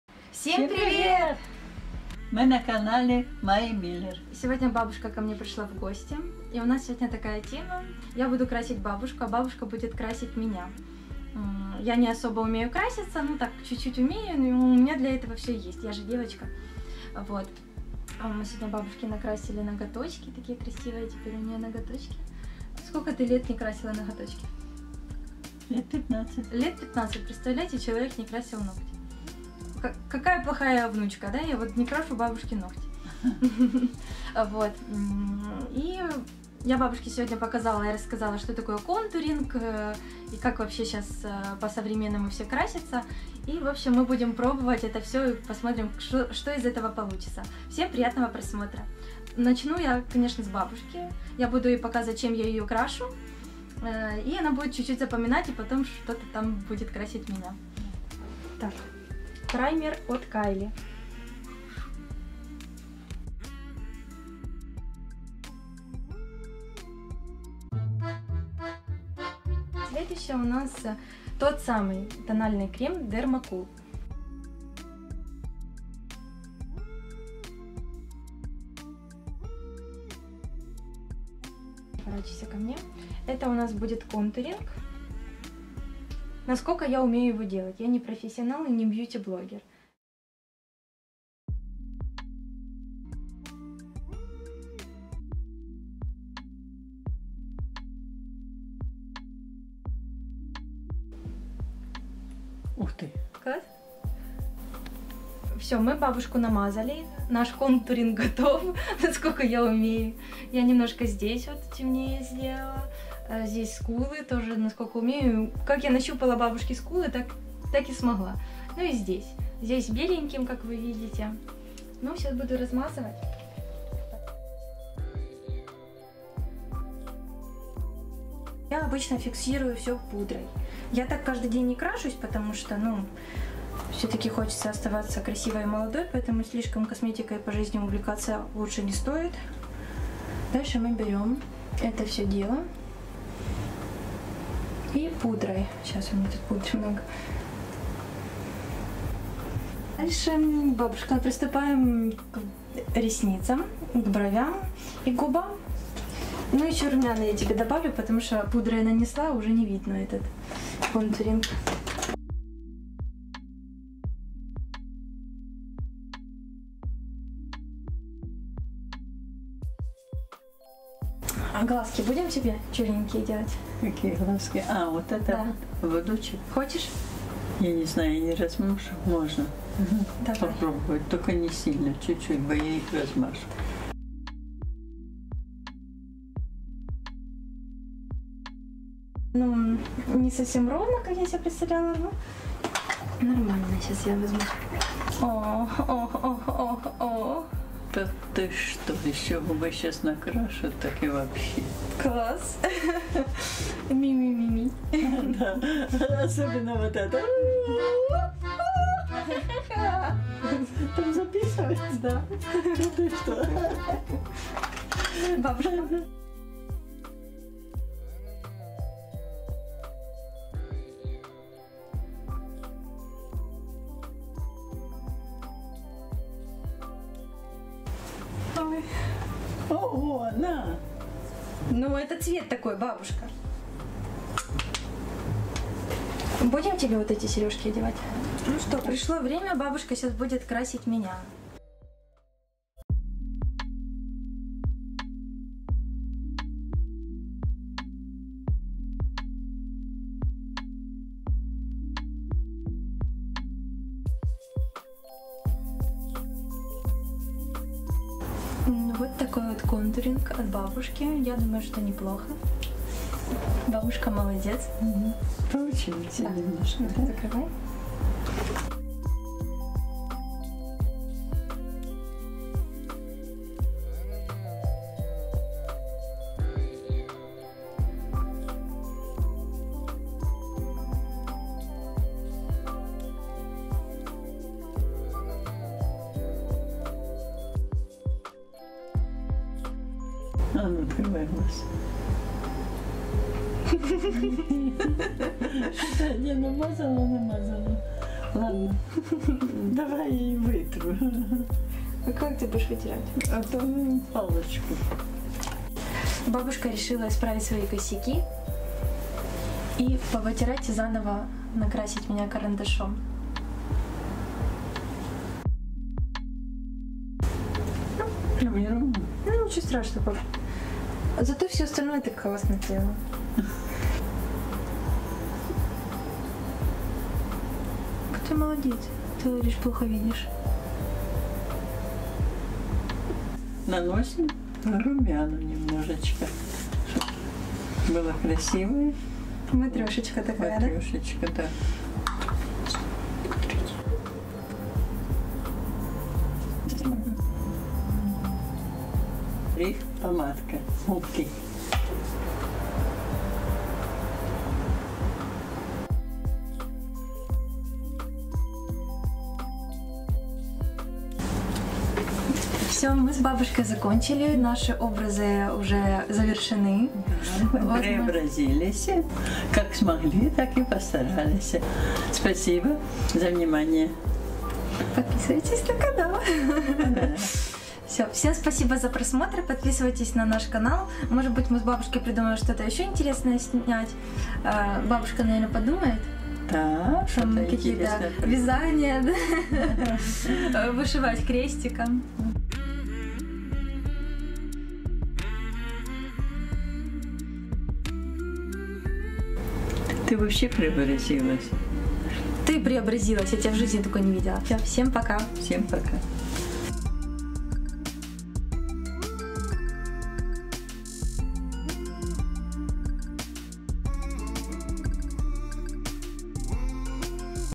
Мейкап от бабушки, крашу бабушку а она меня ))) Веселимся с бабушкой ) make-up from grandma
Мейкап от бабушки, крашу бабушку а она меня ))) Веселимся с бабушкой ).
Моя бабушка не красилась лет 30-40 (только губки)
Ногти не красила лет 15-20.
              Теперь можно зарабатывать не только за показ рекламы)))
               переходи по ссылке и зарегистрируйся.